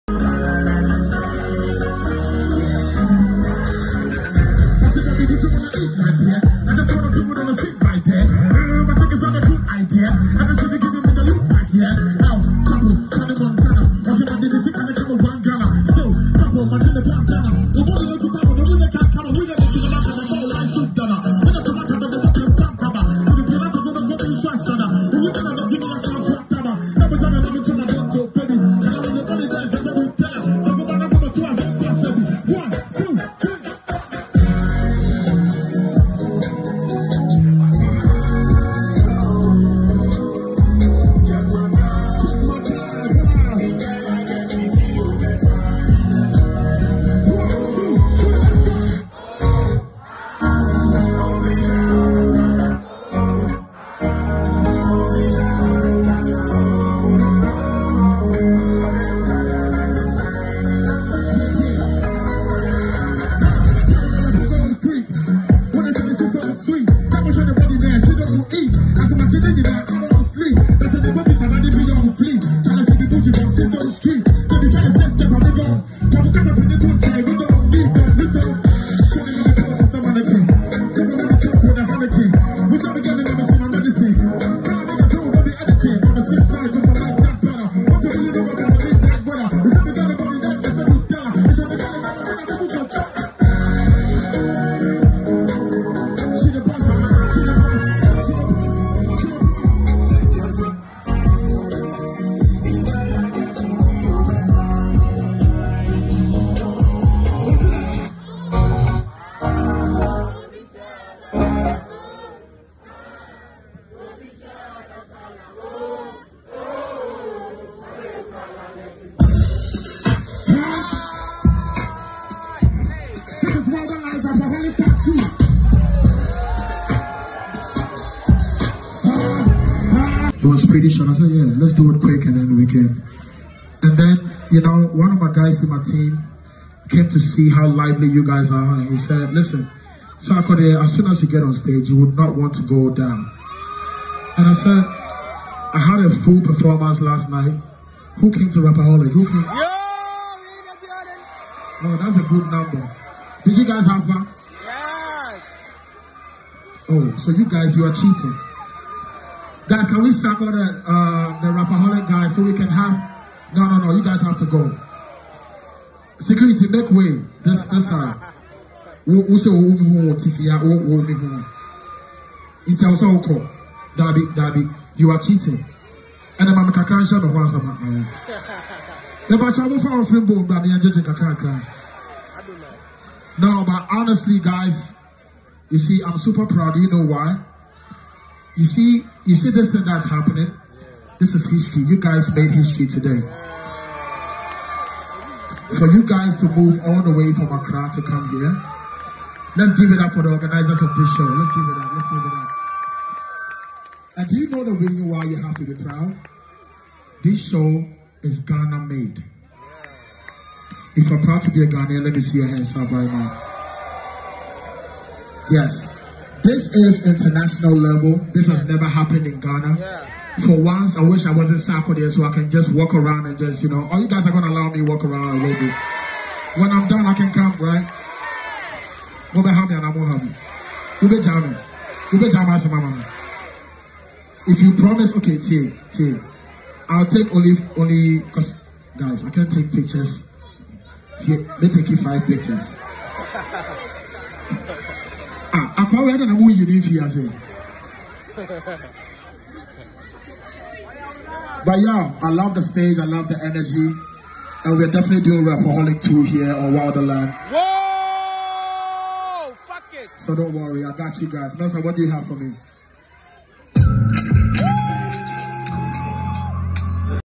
Sarkodie Performance at Wildland Festival🔥🔥
Sarkodie thrilling patrons at Wildland festival